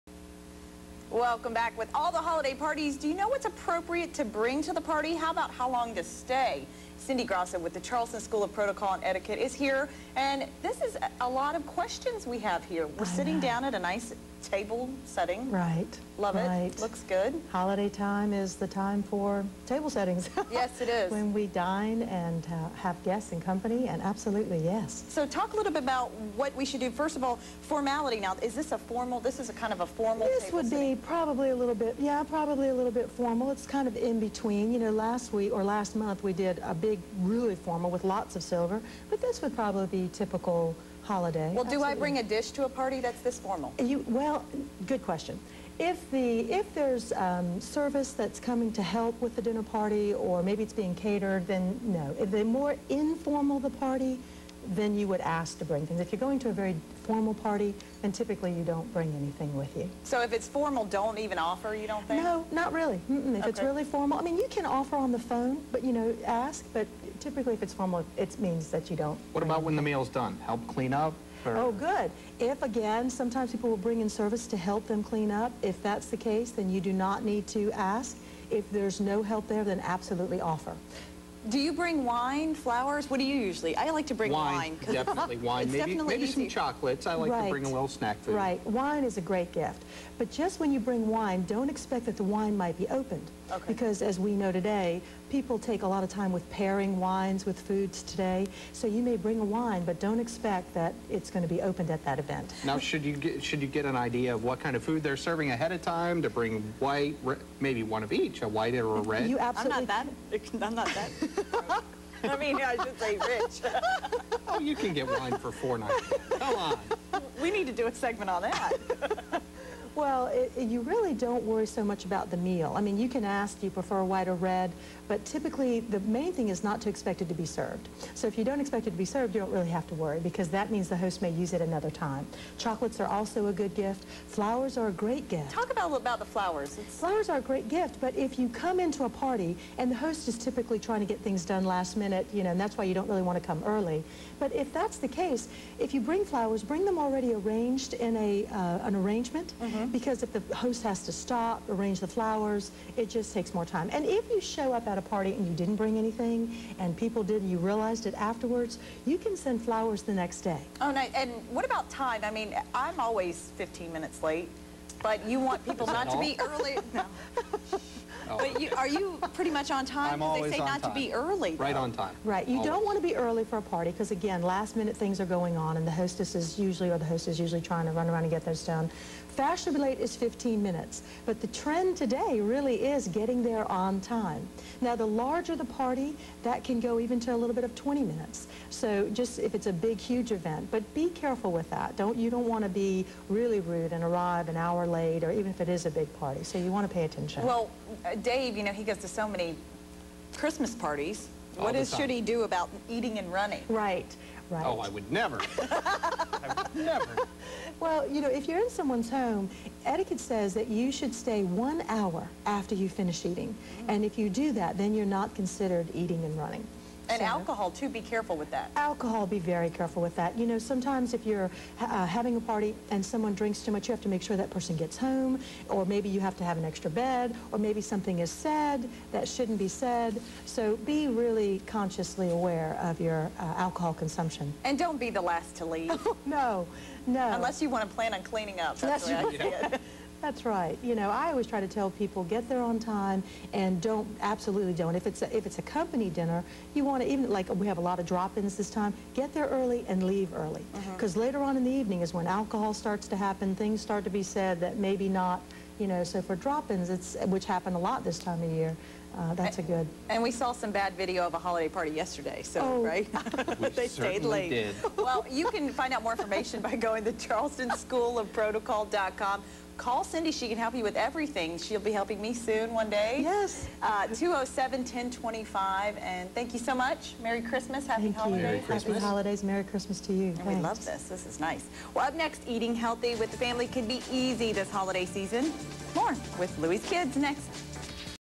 Holiday Etiquette
Cindy talks about the do's and dont's of holiday etiquette.